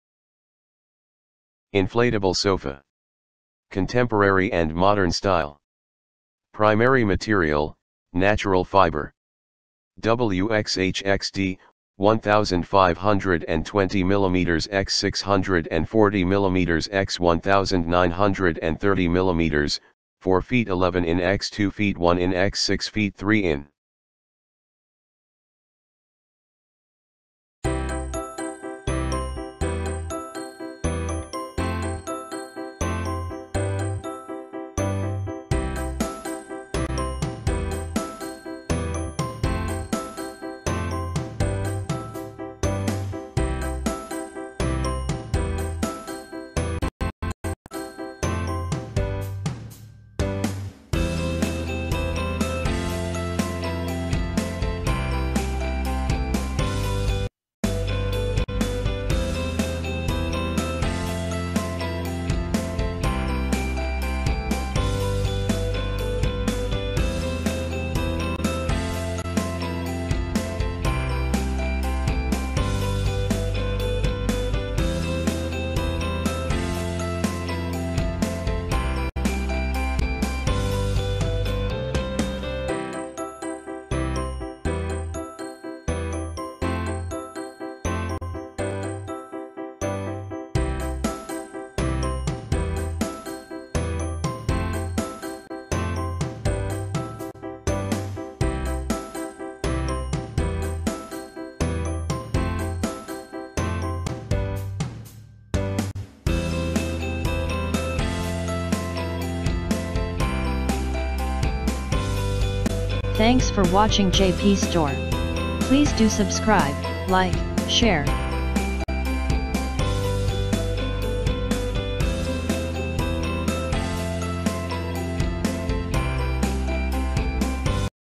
Seaspirit PVC (Polyvinyl Chloride) 3 Seater Inflatable Sofa (Color - Black)
Inflatable Sofa
Contemporary & Modern Style
Primary Material: Natural Fiber
W x H x D: 1520 mm x 640 mm x 1930 mm (4 ft 11 in x 2 ft 1 in x 6 ft 3 in)